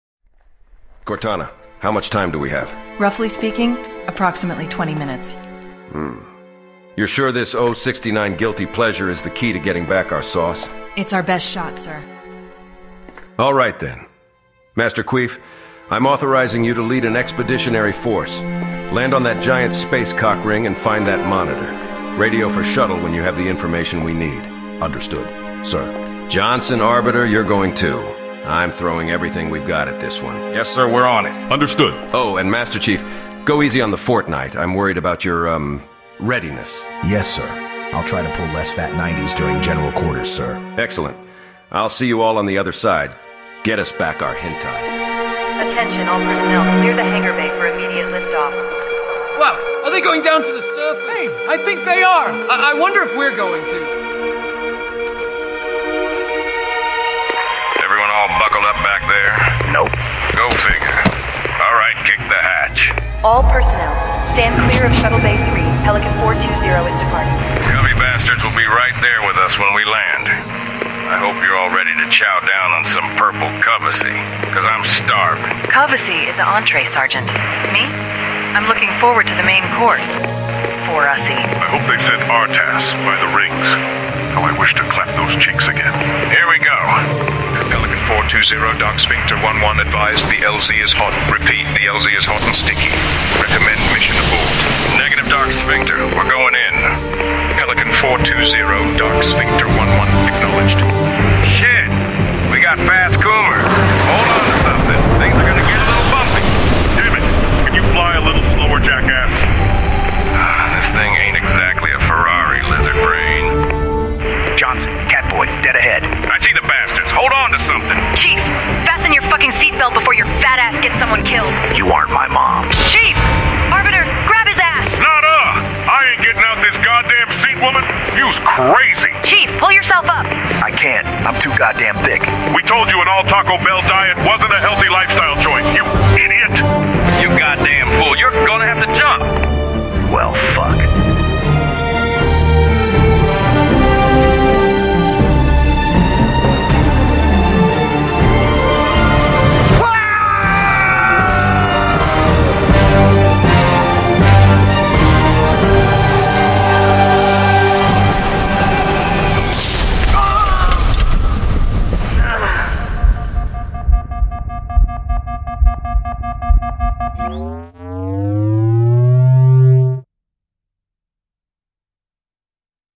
Sauce or Loss with Masterchief: The 3rd
Captain Keyes gives Chief the greenlight on a dangerous mission into uncharted and hostile territory, in a desperate attempt to recover humanities sauce...

Voices provided by Eleven AI using real voice samples